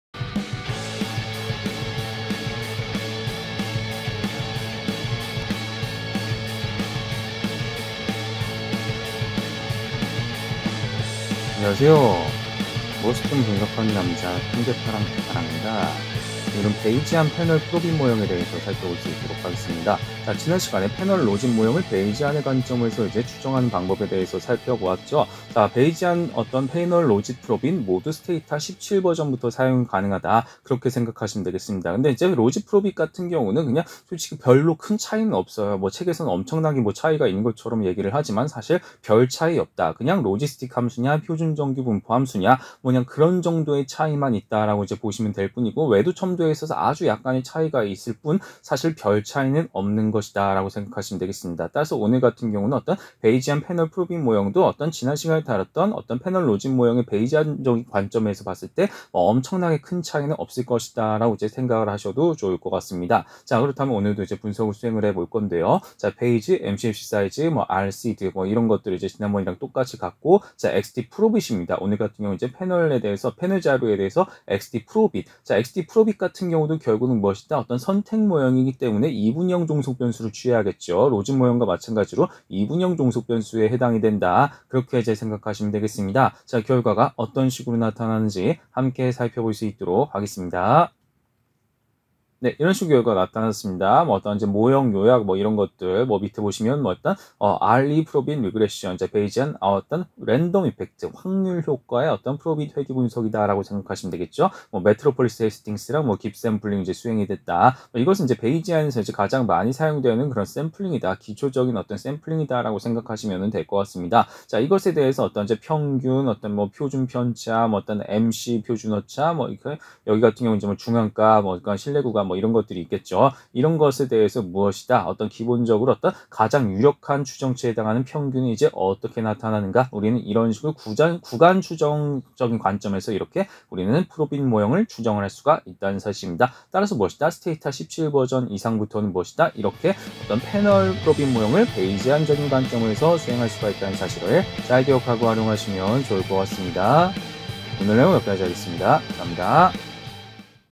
STATA를 활용한 패널분석 - (53) 베이지안 패널 프로빗 모형(Bayesian Panel Probit Model)
베이지안 패널 프로빗 모형(Bayesian Panel Probit Model)

이분형 변수를 대상으로 분석을 수행합니다.